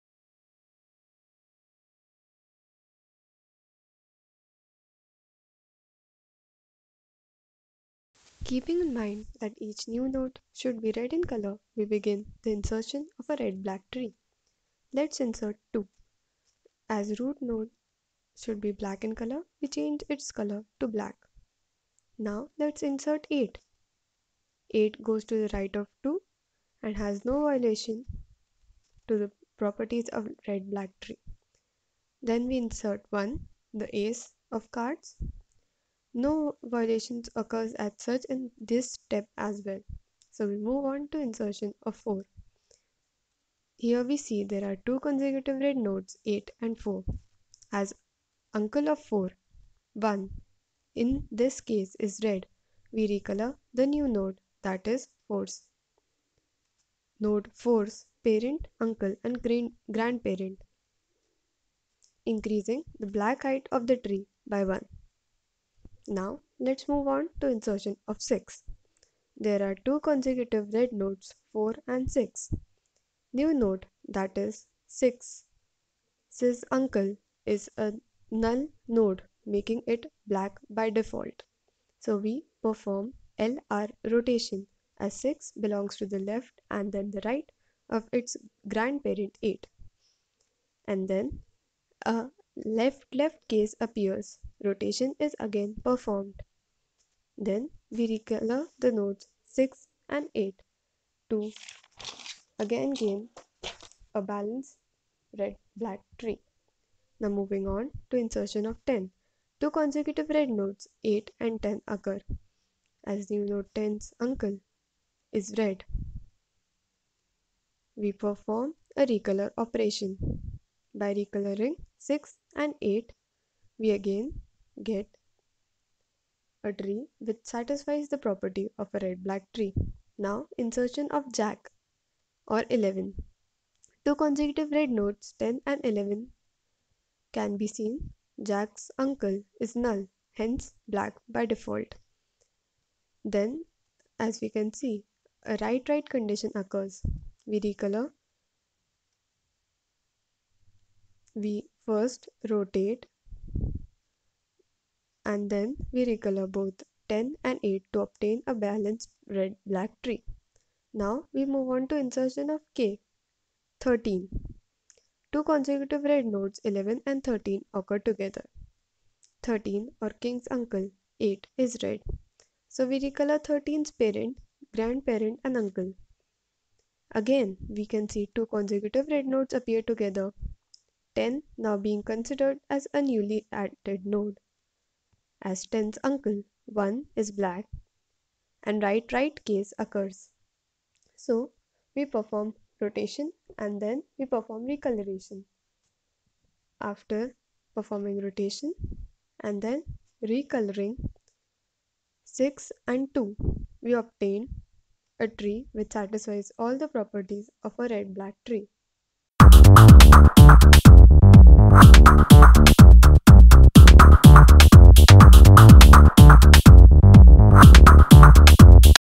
RED BLACK TREE | INSERTION | SOLVED EXAMPLE | DATA STRUCTURE
RED-BLACK TREE is an important data structure. To really get the hang of how insertion happens in a Red-Black Tree we came up with the idea to use playing cards. Playing cards have a lot of advantages as to when one needs to visualize this data structure. Firstly, the 4 suits give red and black color for each number 1 to 13 (i.e. ace to king). Secondly, one doesn't need to get a pen and paper while solving every such problem. It removes the frustration of getting something wrong and increases the chances of learning. Thirdly, the rotations become very easy to visualize and hand movements with the same make it easy to remember. No more drawing unnecessary arrows on direction of rotation in notebook.

A red-black tree is a binary search tree with one extra bit of storage per node: its color, which can be either RED or BLACK. By constraining the node colors on any simple path from the root to a leaf, red-black trees ensure that no such path is more than twice as long as any other, so that the tree is approximately balanced. Each node of the tree now contains the attributes color, key, left, right, and p. If a child or the parent of a node does not exist, the corresponding pointer attribute of the node contains the value NIL. We shall regard these NILs as being pointers to leaves (external nodes) of the binary search tree and the normal, key-bearing nodes as being internal nodes of the tree. A red-black tree is a binary tree that satisﬁes the following red-black properties:
1. Every node is either red or black.

2. The root is black.

3. Every leaf (NIL) is black.

4. If a node is red, then both its children are black.

5. For each node, all simple paths from the node to descendant leaves contain the same number of black nodes.

We add new and amazing videos regularly.
And let us know which type of videos you would like to see next in the comments.